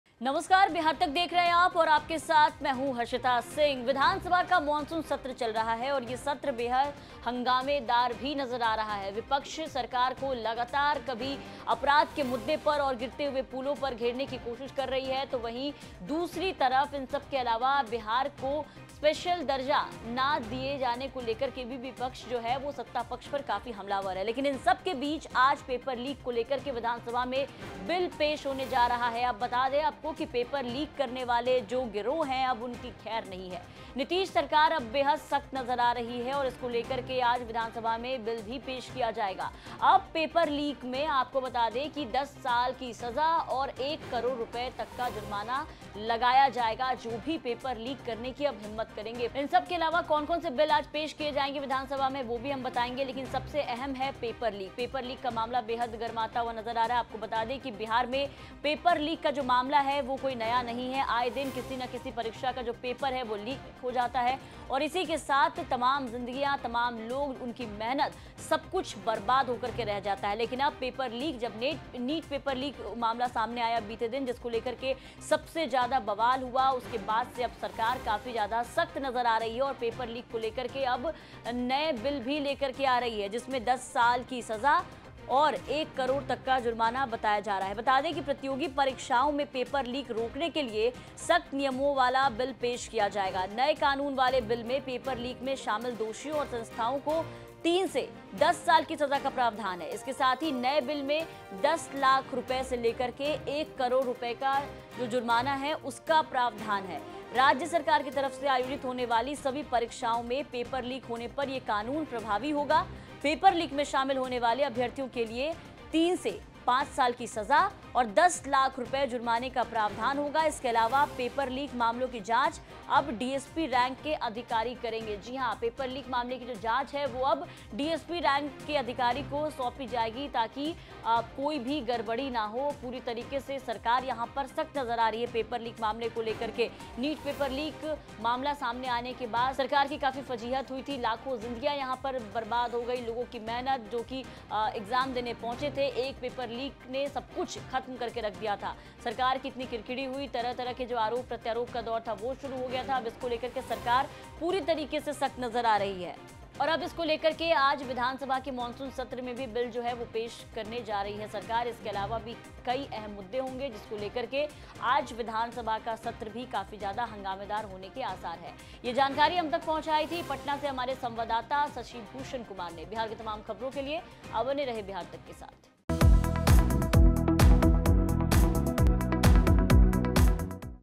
Paper Leak में अब 10 साल की सजा और 1 करोड़ जुर्माना, Vidhansabha में होगा बिल पेश | Bihar Tak
paper leak मामले में सख्त हुई Nitish सरकार पेपर लीक के खिलाफ आज vidhansabha में पेश होगा बिल, 10 साल की सजा और 1 करोड़ जुर्माने का प्रावधान , प्रतियोगी परीक्षाओं में पेपर लीक रोकने के लिए सख्त नियमों वाला बिल पेश किया जाएगा. जानिए कैसे होगी कार्रवाई